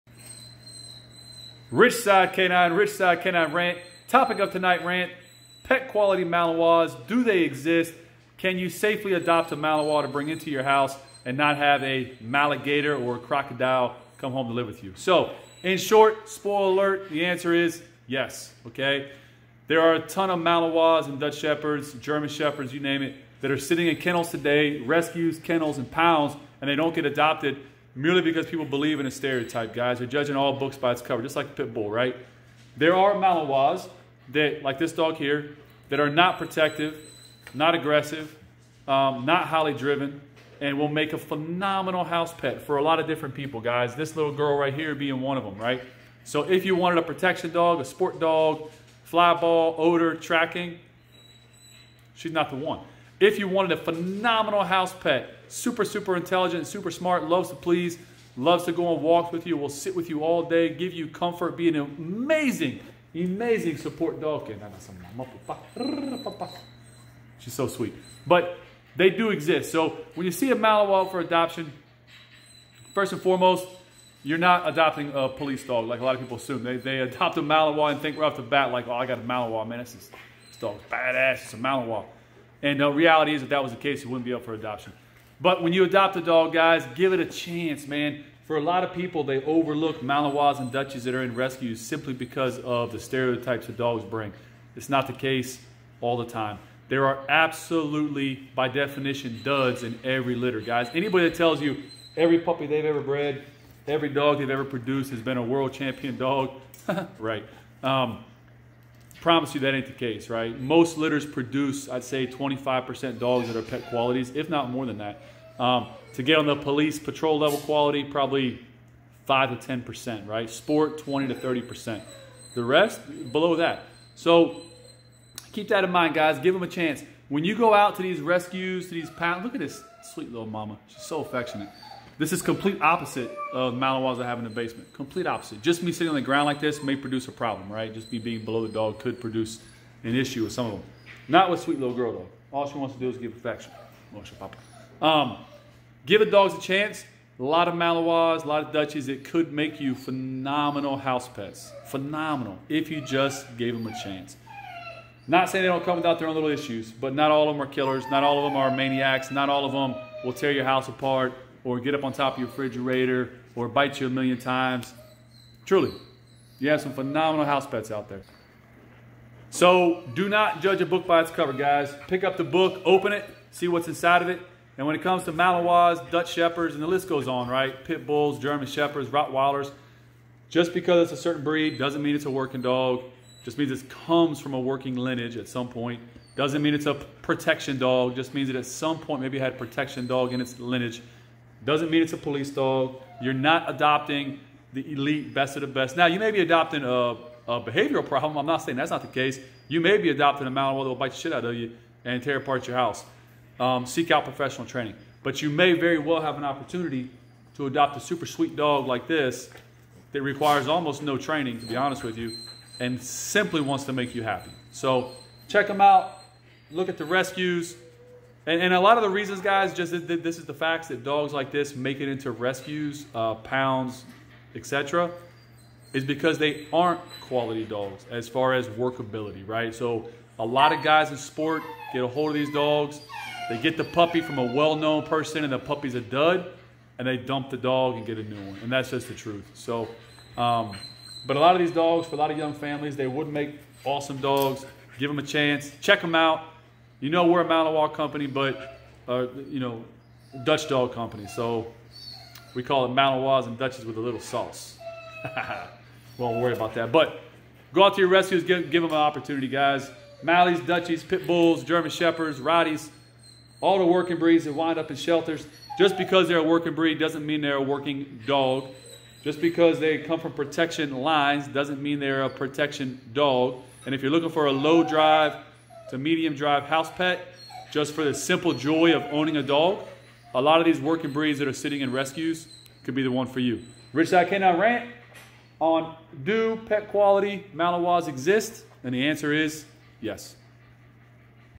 Ridgeside Rant - Do pet quality Malinois exist?
Ridgeside K9 - Rant. Do pet quality Malinois exist?

In this rant I will discuss a topic with a lot of confusion for folks that often call me. I get asked all the time about "buying a Malinois as a family pet".

My answer is simple - DO NOT BUY A PUPPY. Do NOT import a Dutch dog. If your looking for a family pet, seek out a rescue and ask about older dogs that are "proofed" on behaviors. Pet quality Malinois (low drive, non-protective, non-destructive) do exist and are sitting in rescues. Take your time, meet some rescue dogs and give them a shot. Not all Malinois are working dogs, not all are Maligators. Often you hear people say "only get that breed if your a professional" and I agree as far as starting with a puppy. But make no mistake, many amazing house pets get over looked because of the stereotype association with a certain breed.